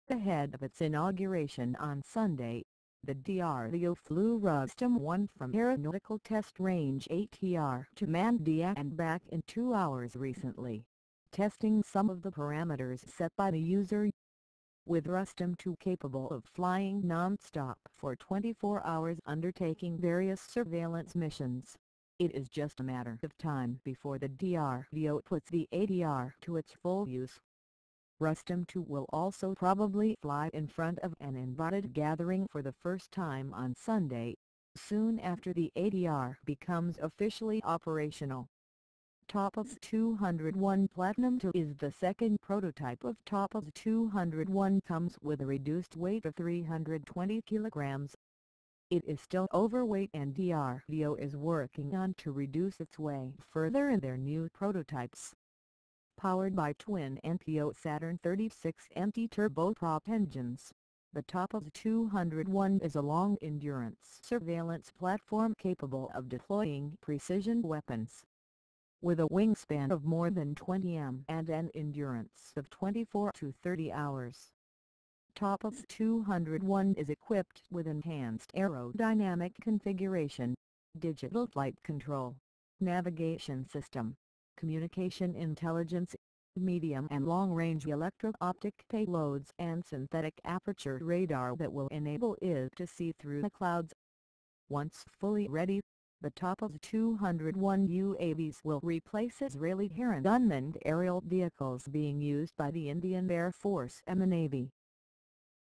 India's Predator Drone TAPAS 201 (Rustom - II )To Test Flight In ATR Inauguration
Ahead of its inauguration on Sunday, the DRDO flew Rustom-1 from Aeronautical Test Range (ATR) to Mandya and back in two hours recently, testing some of the parameters set by the user. With Rustom-2 capable of flying non-stop for 24 hours undertaking various surveillance missions, it is just a matter of time before the DRDO puts the ATR to its full use.
Rustom-2 will also probably fly in front of an invited gathering for the first time on Sunday, soon after the ATR becomes officially operational.
TAPAS 201 PT-2 is the second prototype of TAPAS 201 comes with a reduced weight of 320 kgs. It is still overweight and DRDO is working on to reduce its weight further in their new prototypes.
Powered by twin NPO-Saturn 36MT turboprop engines, the TAPAS 201 is a long endurance surveillance platform capable of deploying precision weapons. With a wingspan of more than 20m and an endurance of 24 to 30 hours.
TAPAS 201 is equipped with enhanced aerodynamic configuration, digital flight control, navigation system, communication intelligence, medium and long-range electro-optic payloads and synthetic aperture radar that will enable it to see through the clouds.
Once fully ready, the TAPAS 201 UAVs will replace Israeli Heron unmanned aerial vehicles being used by the Indian air force and the navy.